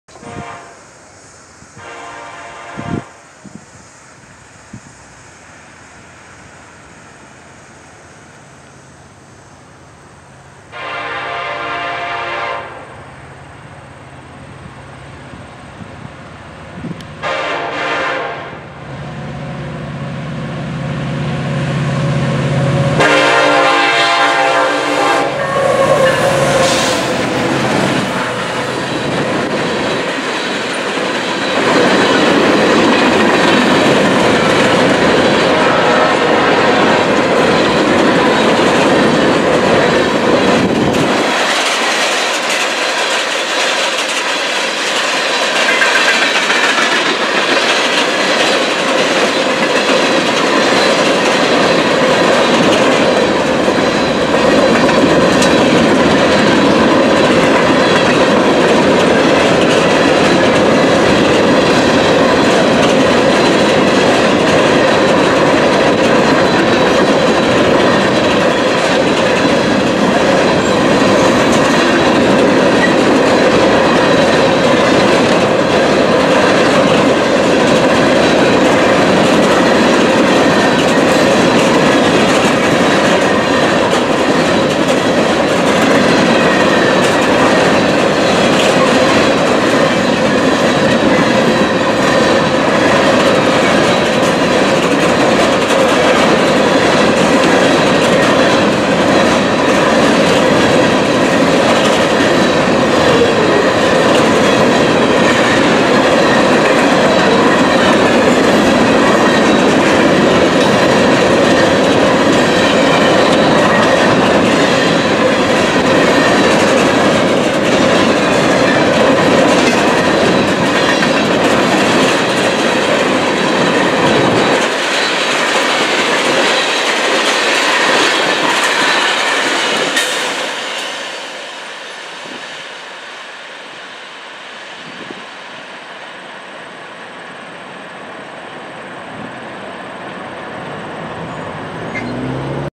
9/2/20 Kendallville, IN; BNSF 9043 leads a coal train
BNSF 9043 SD70ACE and BNSF 9044 SD70ACE take an empty coal train west at 4:47 P.M.